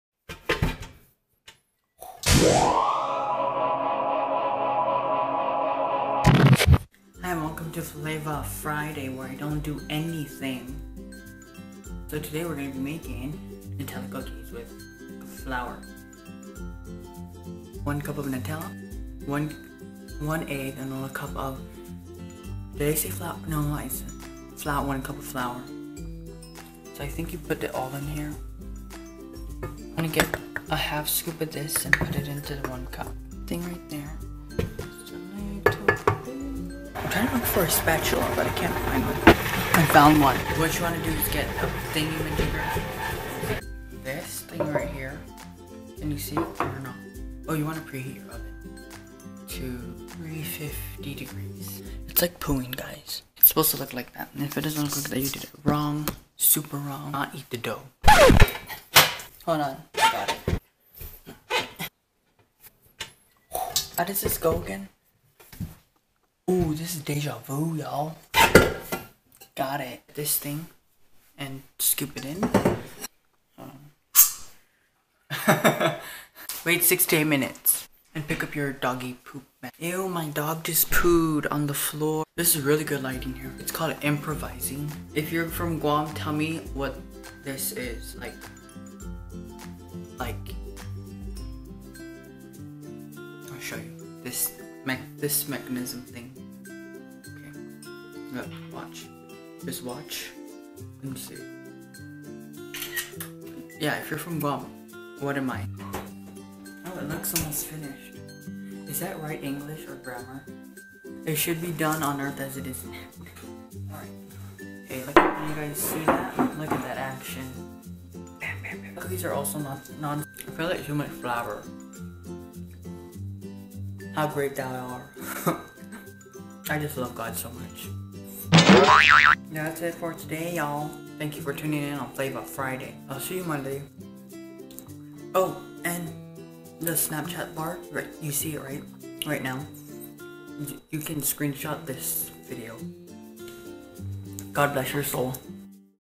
flava friday episode 1//nutella cookies///chad salas
for this you will need 3 ingredients obviously
1 Egg
1 Cup of flour
1 Cup of nutella